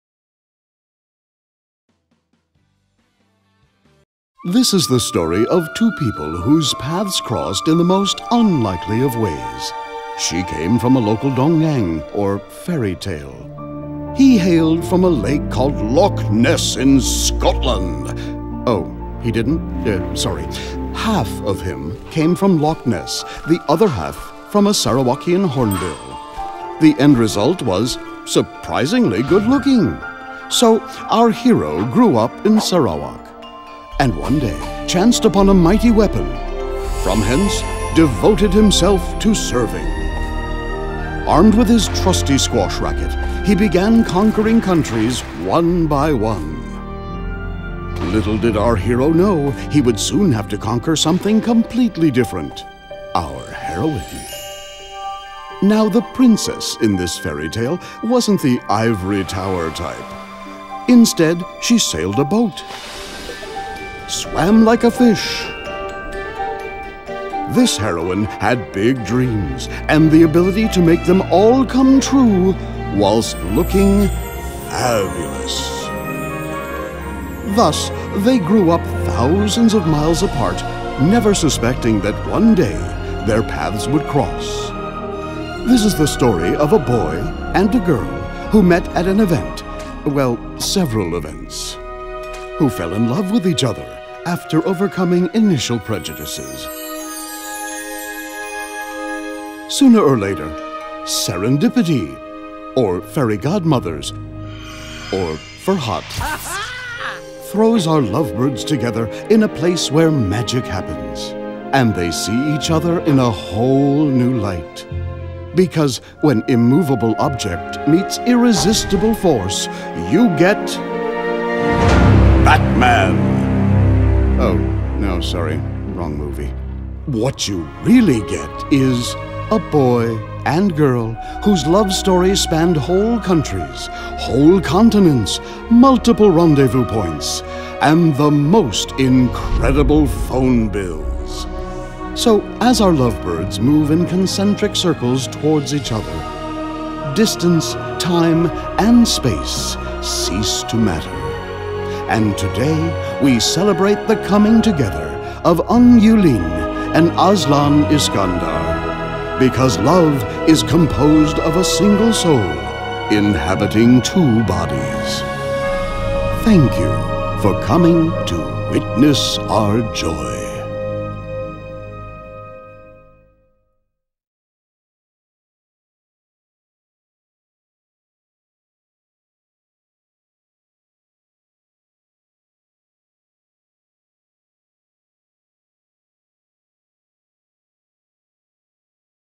Azlan Iskandar & Ung Yiu Lin Short Animation Feature
When Malaysia's World Class Squash Athlete, Azlan Iskandar and Designer/Entrepreneur Extraordinaire, Ung Yiu Lin "tied the knot" together, they wanted to produce a short that told their own, unique story, in a extraordinary way. Shiroku was given the opportunity to create an animated story that revisits the old fantasy genre. Working with the design and creative team, Nicky Tan, the director of the feature, developed a look & feel for the piece that was both memorable and mythic-past inspired. The "Tim Burton-esque" story of how the couple met and fell in love was shown during their wedding dinner. All the best and congratulations to Azlan & Yiu Lin for many happy years together!

Executive Producer: Peter Chin
Director: Nicky Tan
Producer: Imee Kamalia
Creative Director: Jay Sheldon
Writer: Catherine Ooi
Design Lead: Chai Yen
Animation/Motion Design team: Nicky Tan, Yoke Wan, Carmen Chan Jia Myn
Audio House: Chris Higgs
Voiceover: Jay Sheldon